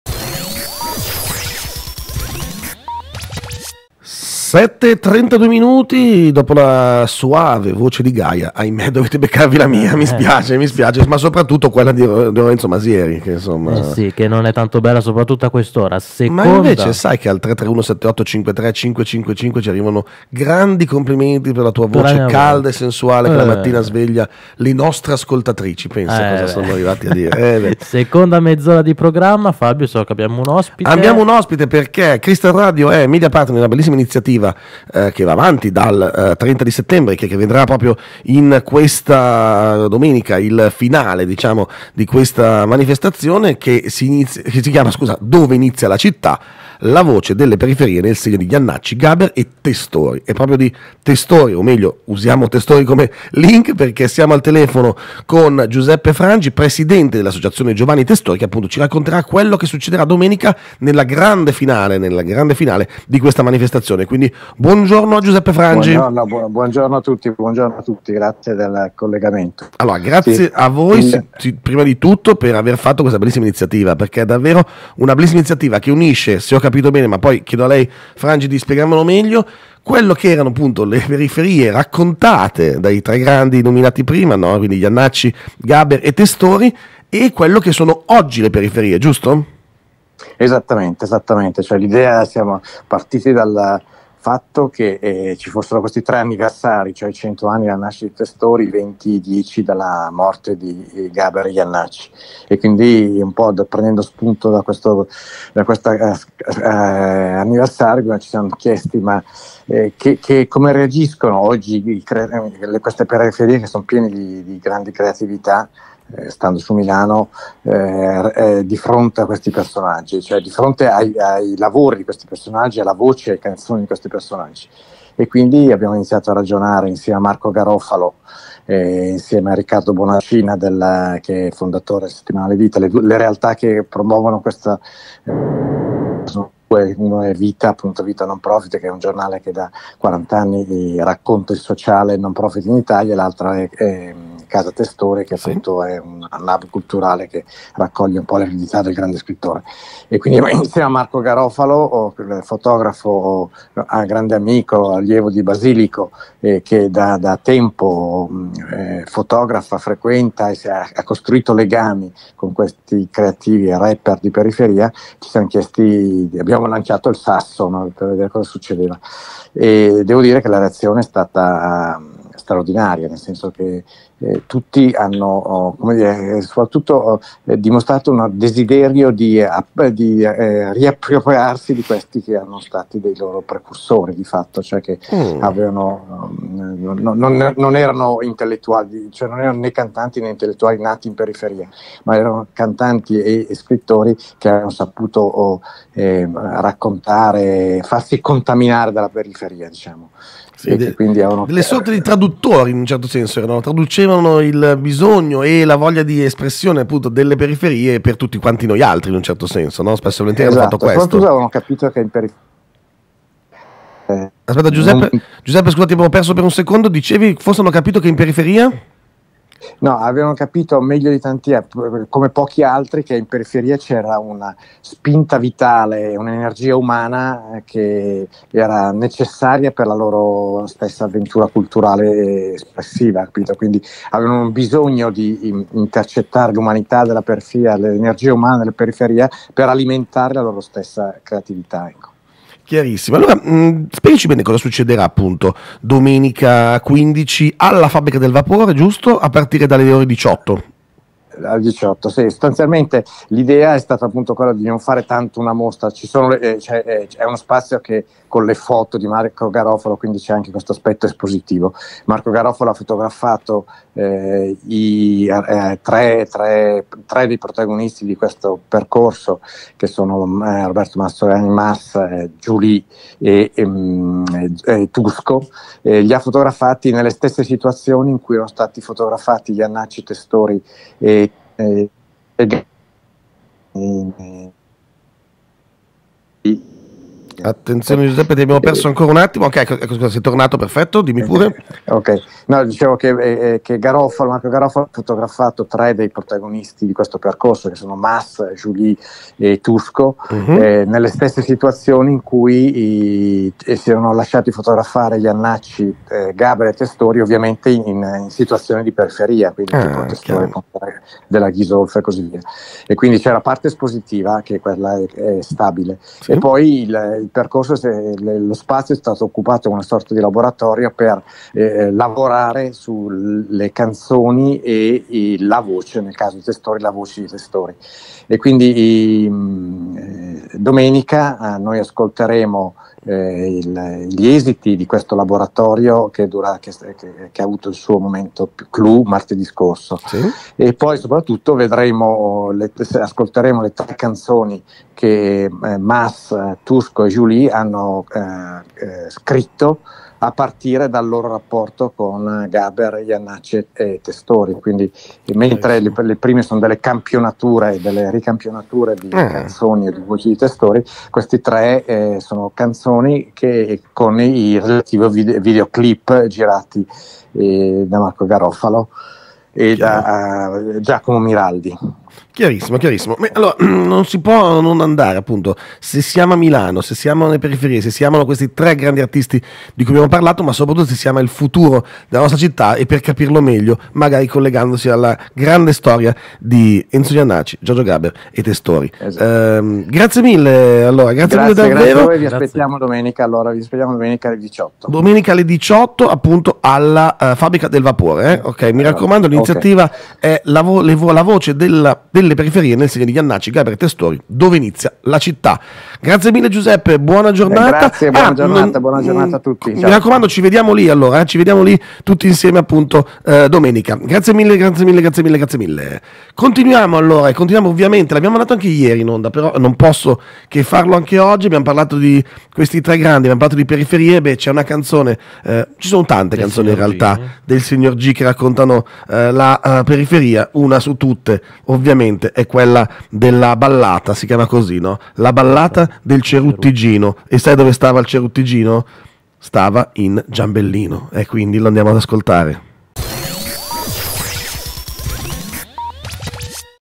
GoodMorning Milano - Giuseppe Frangi
In collegamento con Giuseppe Frangi nella nuova puntata di GoodMorning Milano.